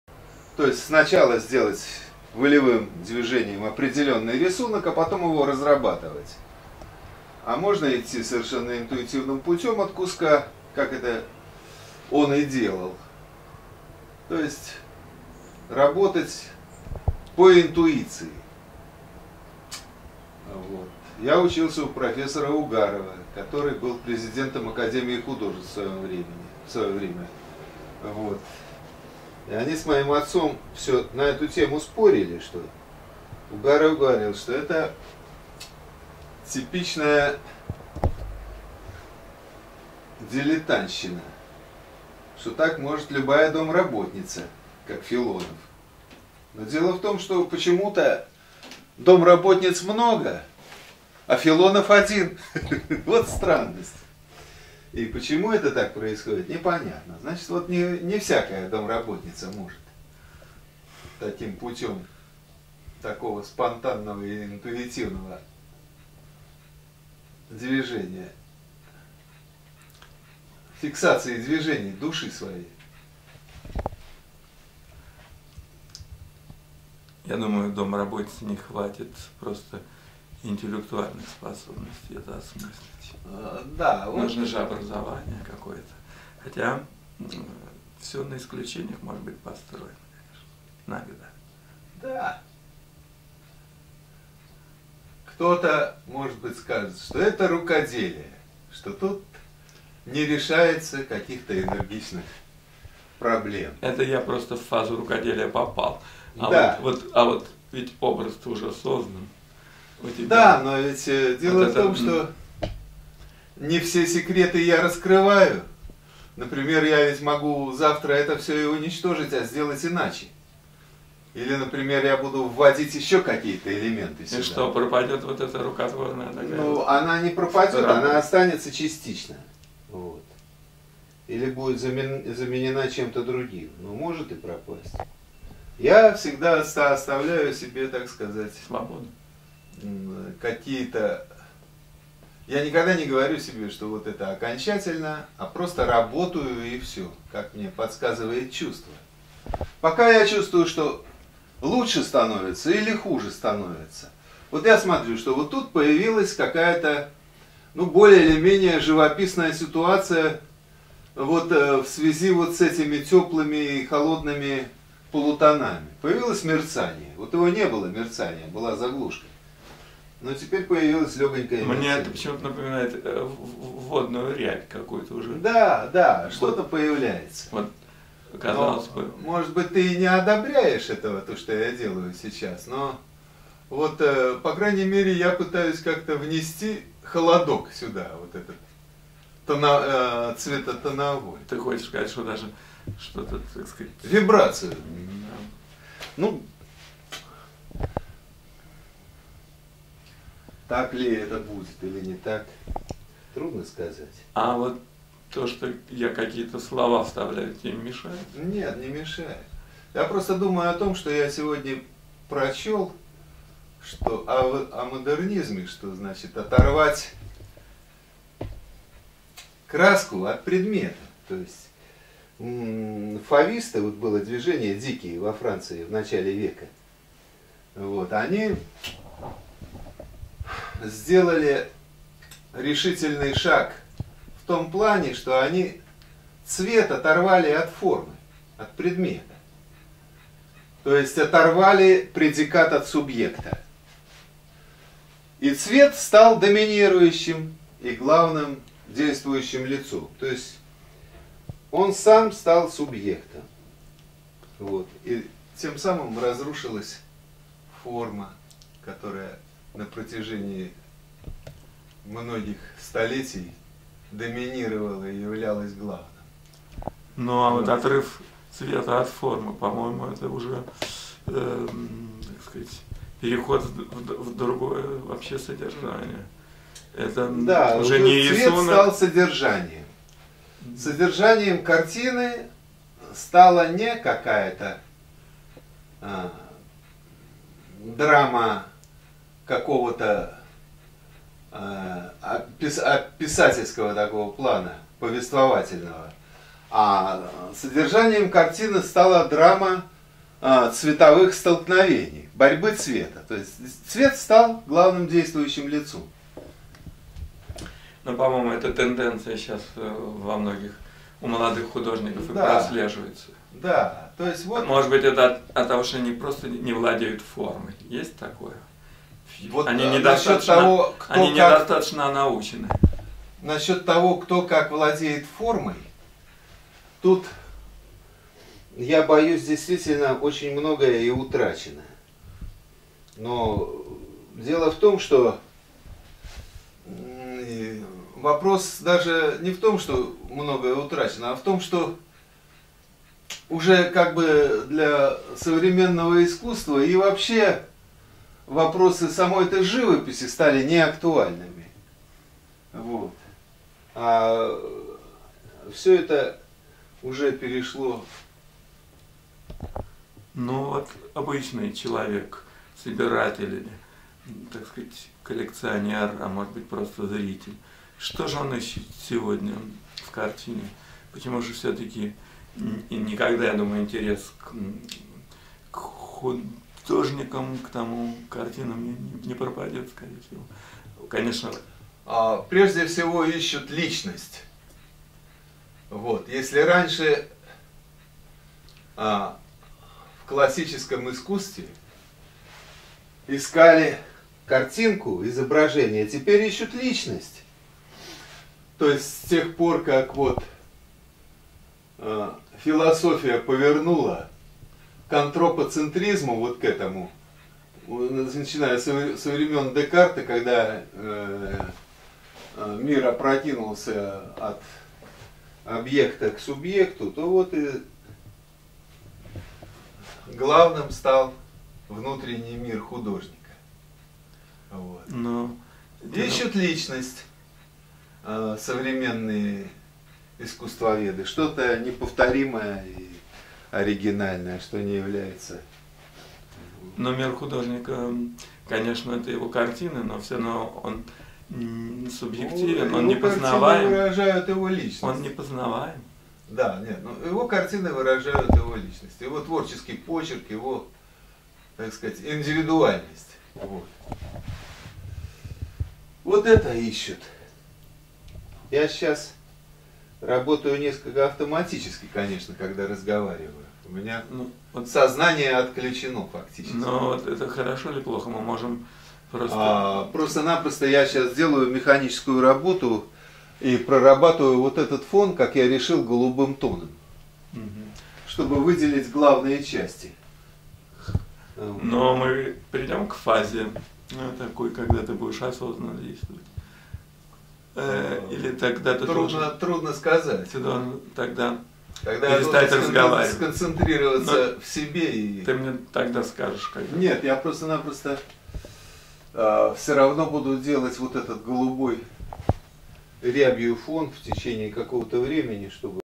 кубизм стиль художника Александра Альховского Lessons1
Künstler Alexander Alkhovski und modernen Malerei, modern painting. Lessons1 Лекции о живописи в прочтении петербургского художника Александра Албховского. Современная живопись это драма цветовых столкновений и разрушение формы. Цвет стал оторванным от формы. Цвет стал доминирующим субъектом. Вопросы живописи стали неактуальны. Со времен Декарта мир пережил переход от объекта к субъекту.